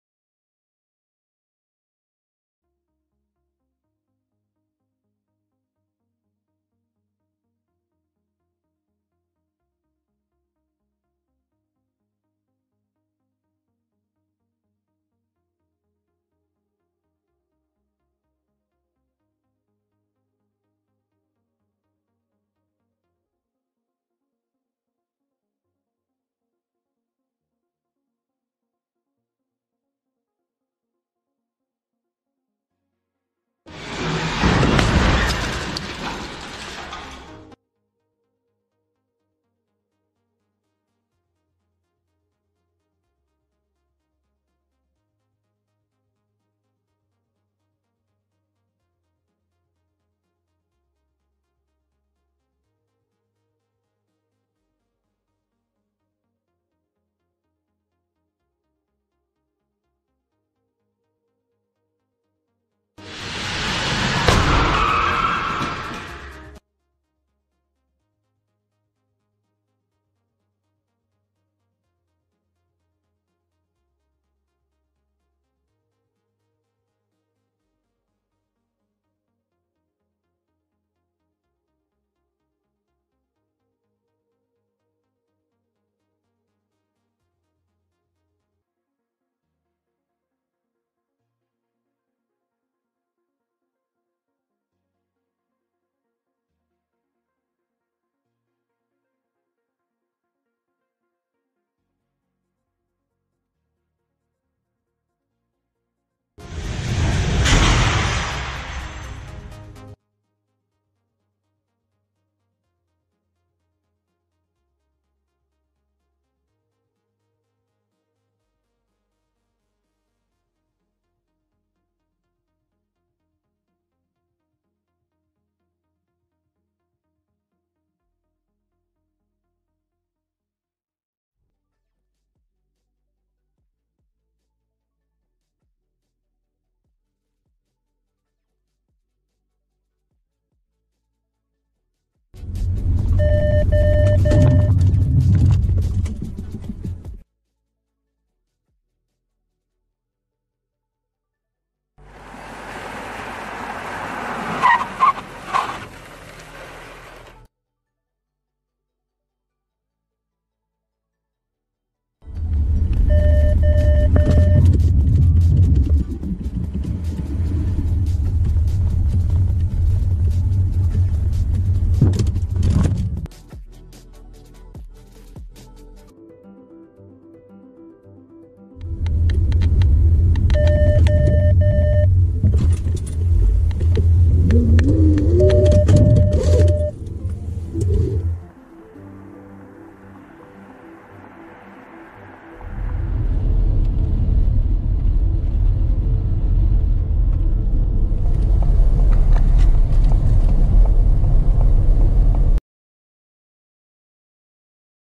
2021款 福特 野马 Mach-E 澳洲车辆测试机构 全面测试
2021款 福特 野马 Mach-E 澳洲车辆测试机构 全面测试

 机械,汽车,测试,澳大利亚,野马,福特